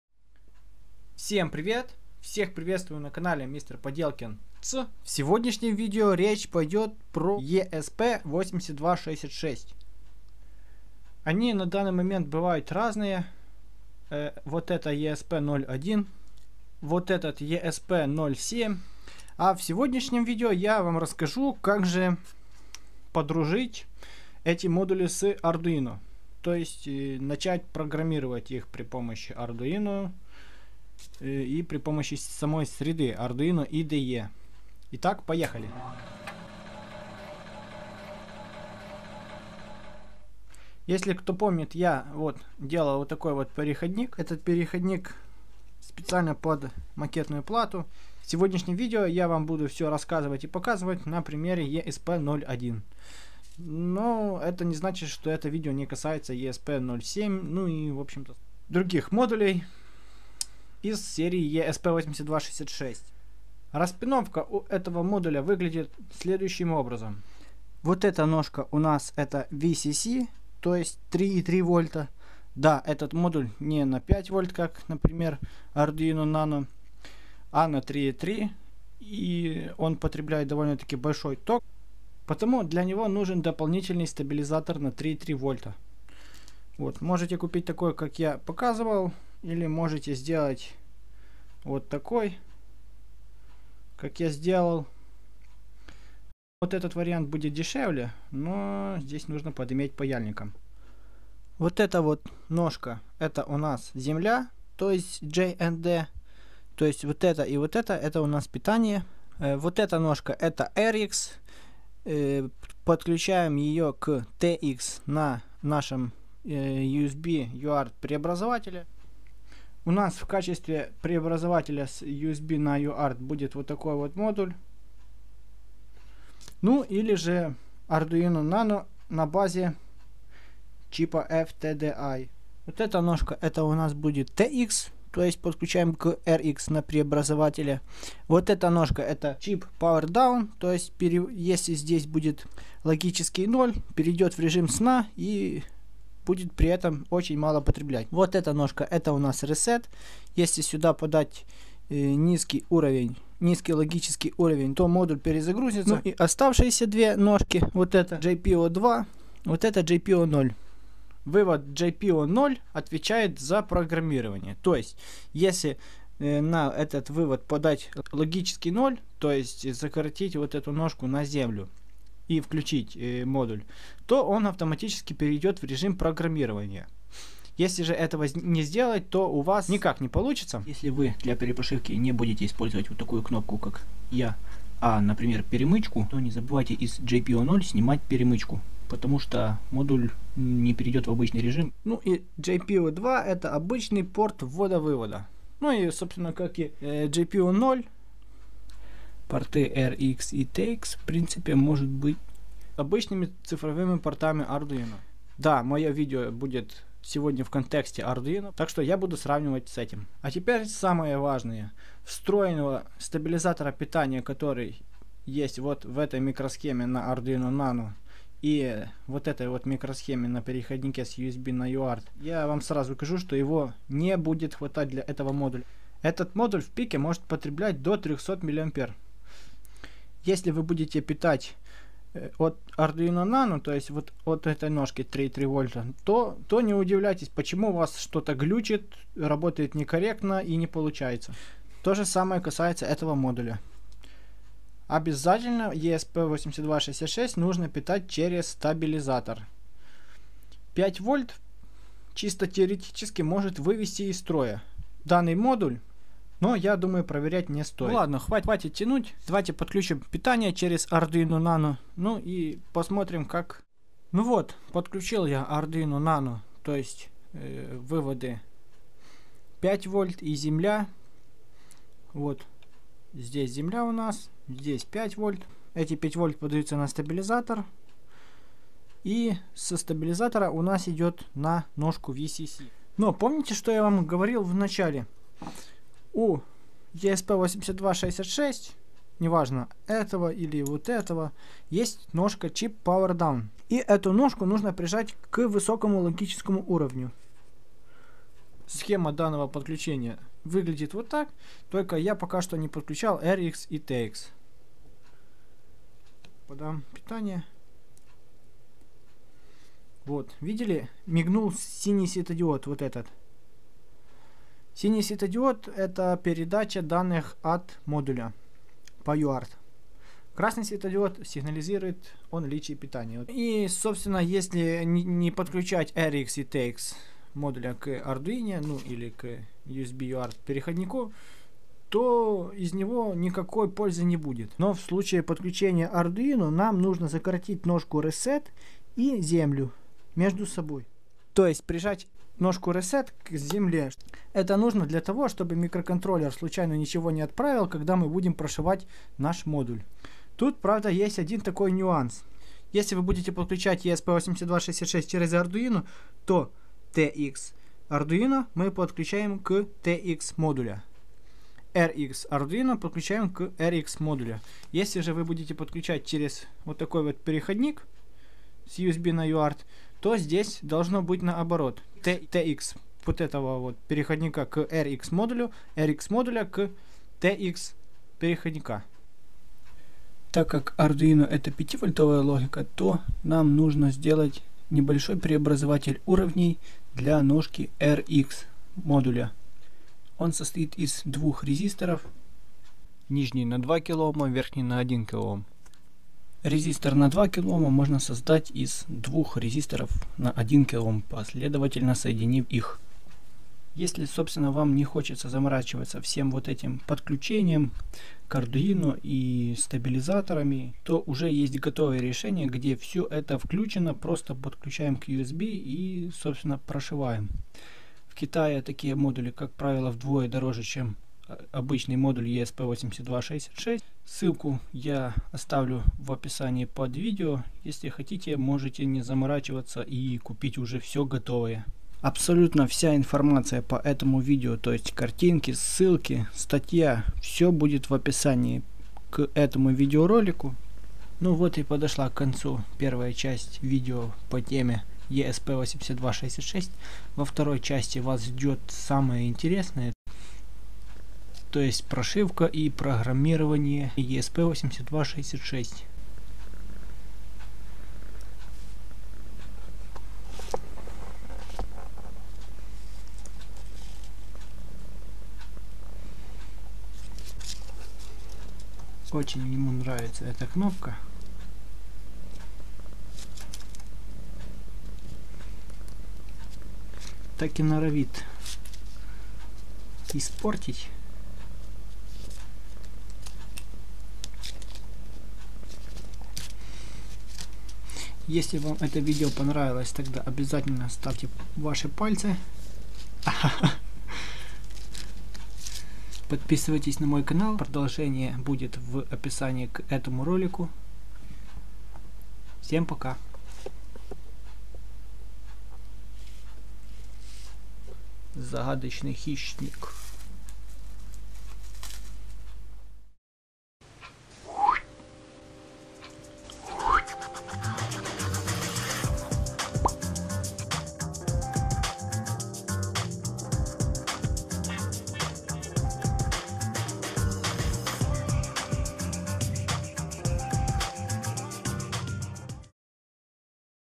ESP8266 и Arduino, подключение, распиновка
Вторая часть статьи "ESP8266 и Arduino IDE, прошивка и программирование"

В данном видео я затрону такую тему, как подключение WiFi модуля ESP8266 к Arduino. В частности я покажу как подключается ESP01 и Arduino Nano V3, или как вообще можно обойтись без ардуино. Так же я покажу распиновку модуля и расскажу какая ножка за что отвечает. Подготовлю вас к теме прошивки и программирования модуля в среде разработки Arduino IDE, так сказать. Чтобы потом возникало меньше вопросов. Все подробности в статье, ссылка на которую выше.